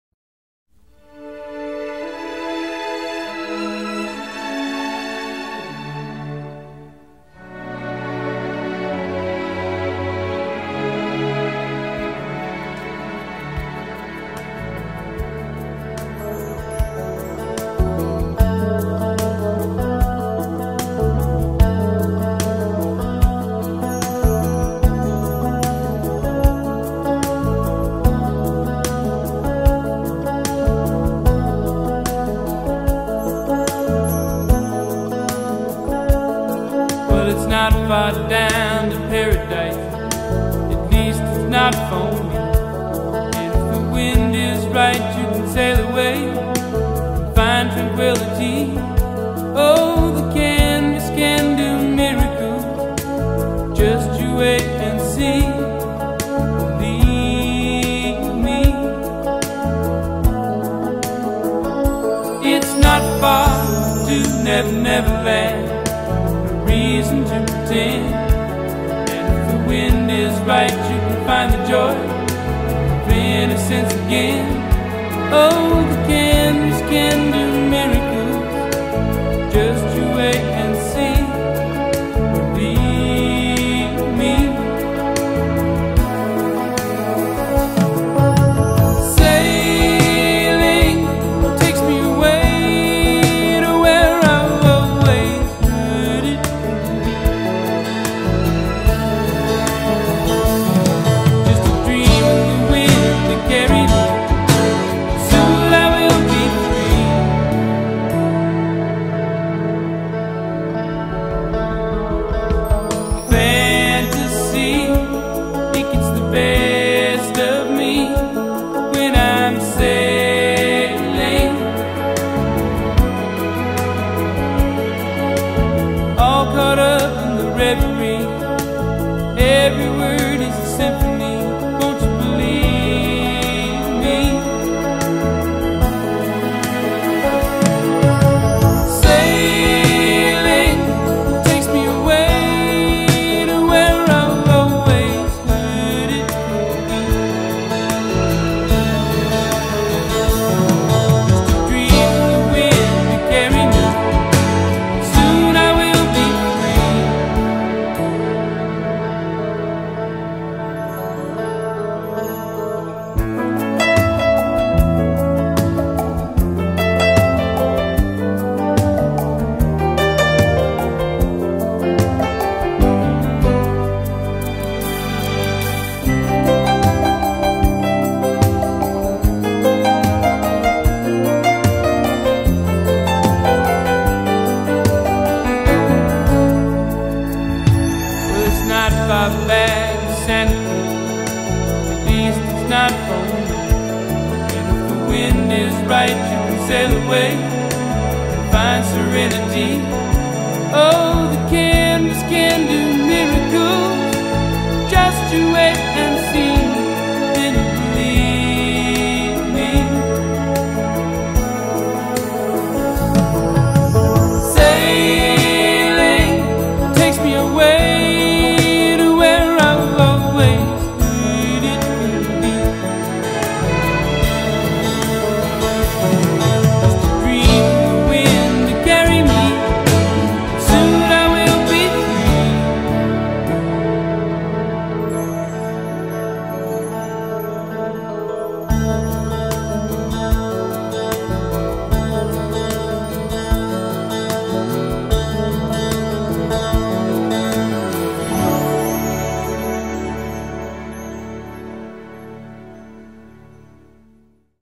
Mom's funeral slide show #1
A slide show to one of my mom's favorite Christopher Cross songs. I tried to stay in time date order with the photos.